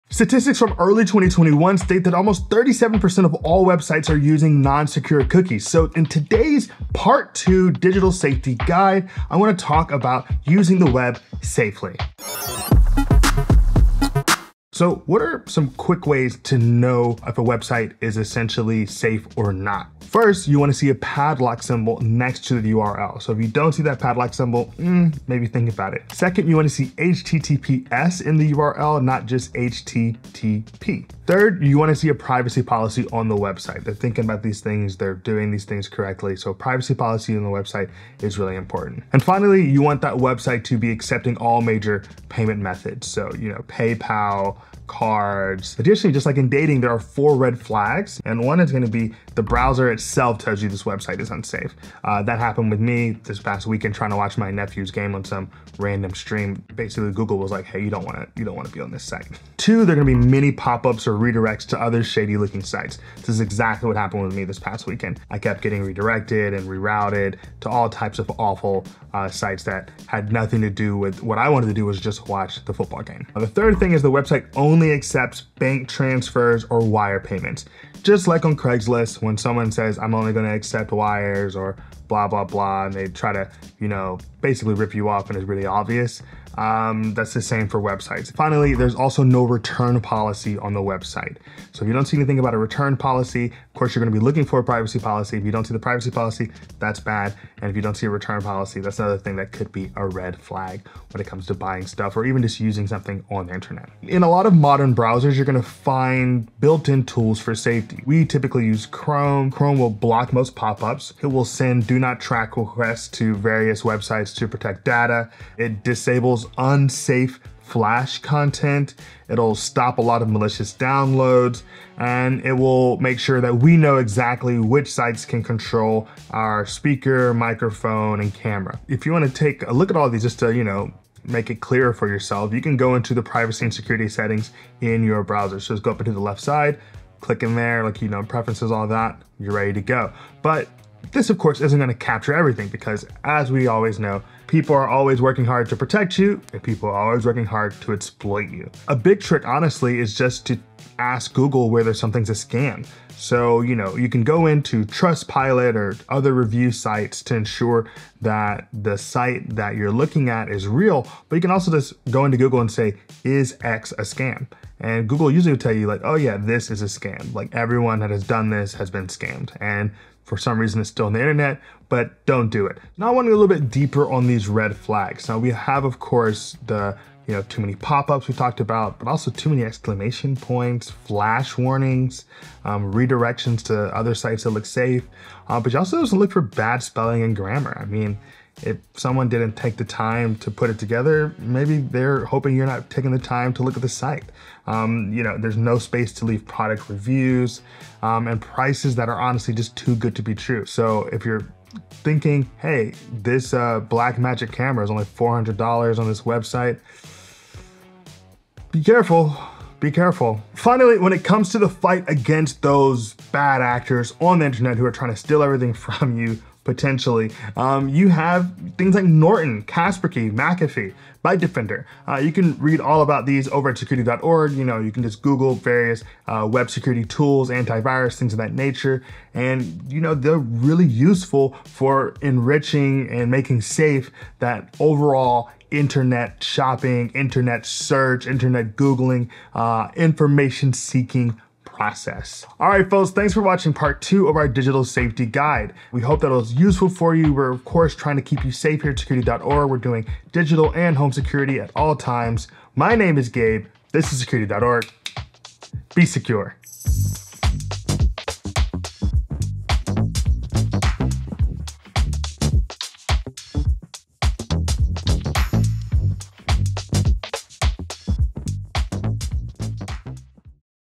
How To Use The Web Safely | Digital Security Guide Part 2
To learn more about how to use the web safely, check out our complete digital safety guide

Gabe discusses the basics for using the web safely in part two of our three part series on digital security & online safety.

00:00 - 37% of all websites use non-secure cookies
00:12 - Quick ways to determine if a website is safe
00:46 - Red flags to watch out for regarding website safety
01:44 - Browser safety tools
02:09 - View your privacy and security settings
02:52 - Other things to look for
03:30 - Additional safety tools